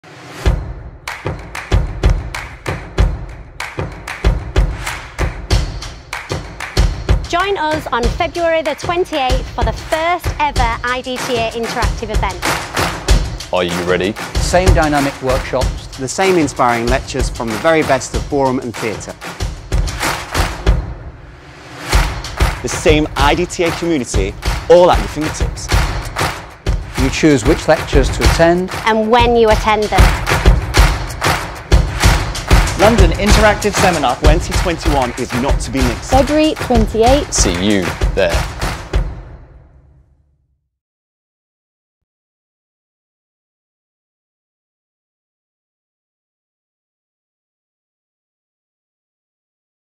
IDTA London Seminar Online 2021
WE ARE GOING INTERACTIVE 🌐
For the first time the IDTA London seminar will be online and interactive.
Select which lectures you attend and when you attend them. 📲🖥💻📺
Booking now open ⬇
Even online the IDTA is truly at the heart of dance 💜
Use idtalondon2021 to share your posts with us. Will you be having a virtual meet with dance friends? What country will you be joining us from?
Let us know we want to hear from you